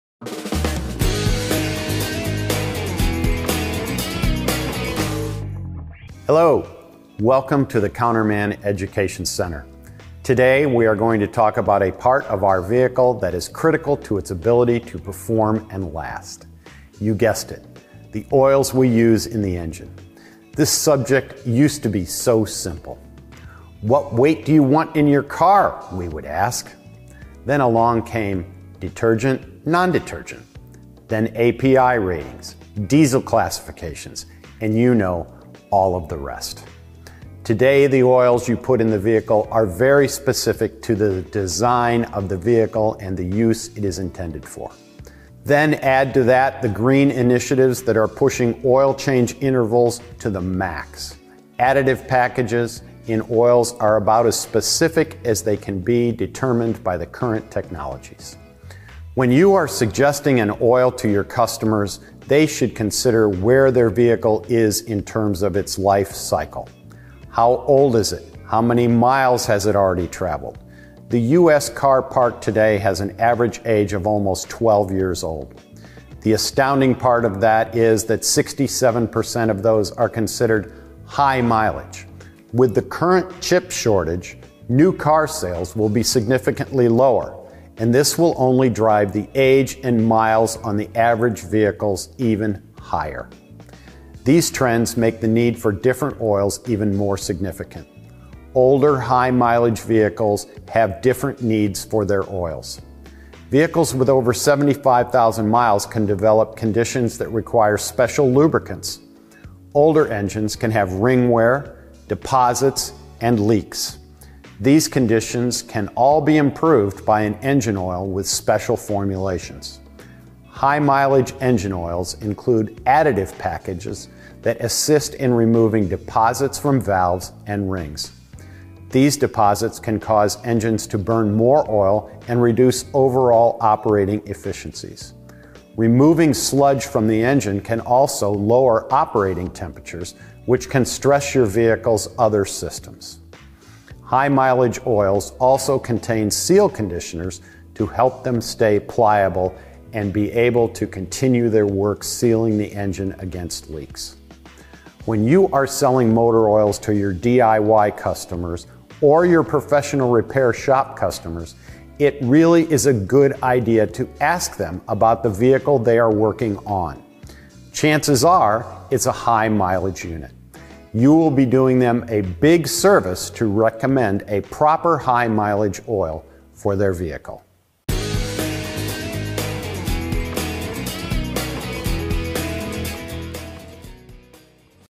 High-Mileage Motor Oils
The subject of motor oil used to be simple. Today, the oils you put in the vehicle are very specific to the design of the vehicle and the use for which it’s intended.